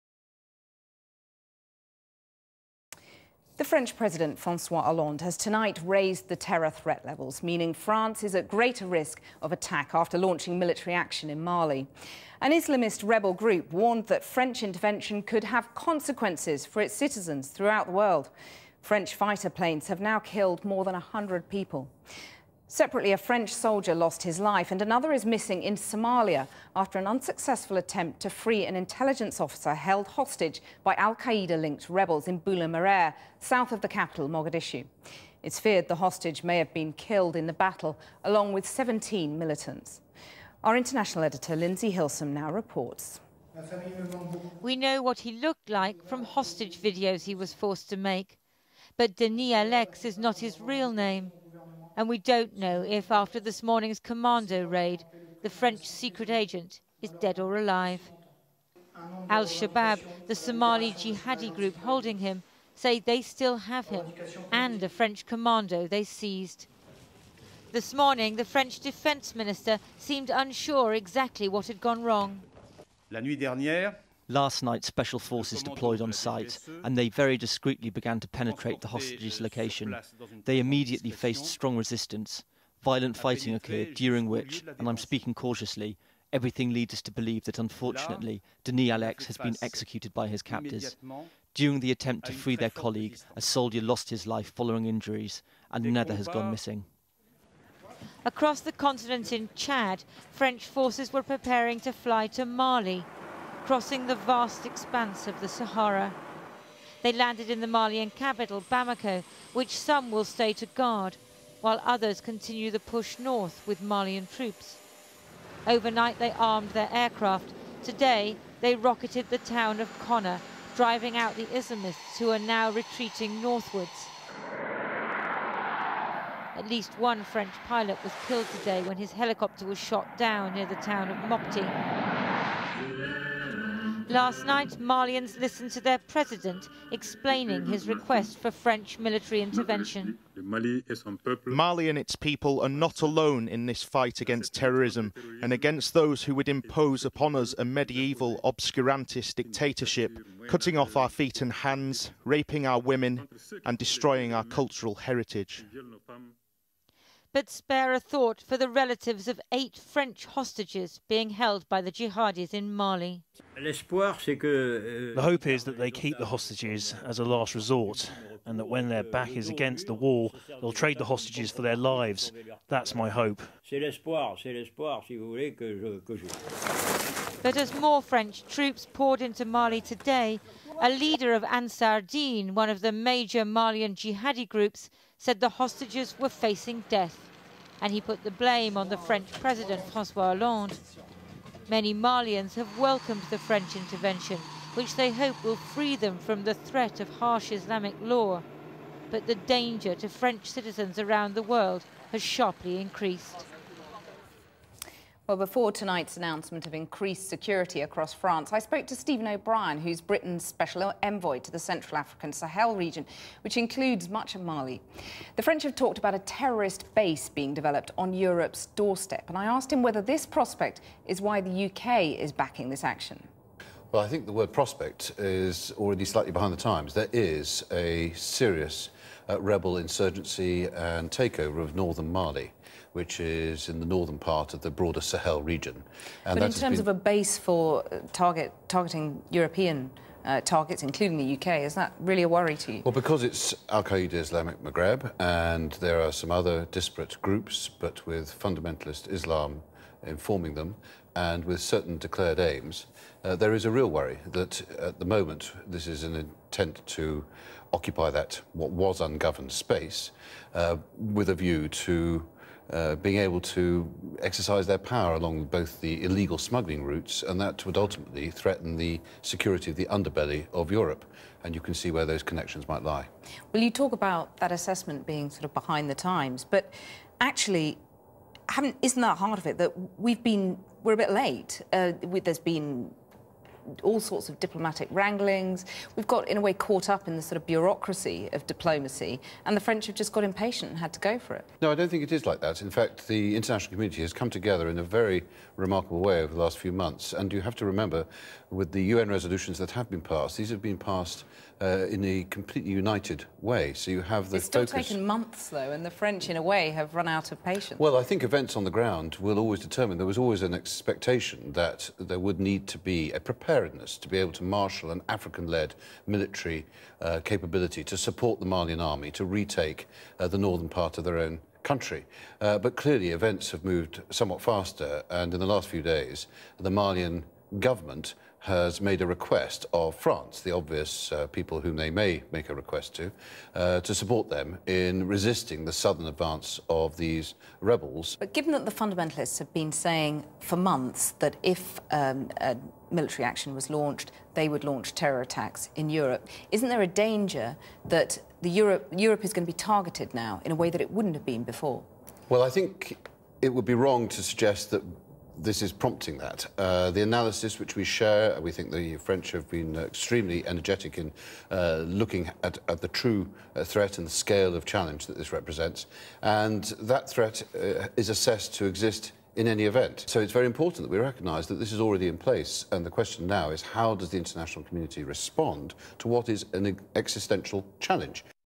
France under terror threat after 'failed hostage rescue'
The French president, Francois Hollande, has tonight raised the terror threat levels - meaning France is at greater risk of attack after launching military action in Mali. .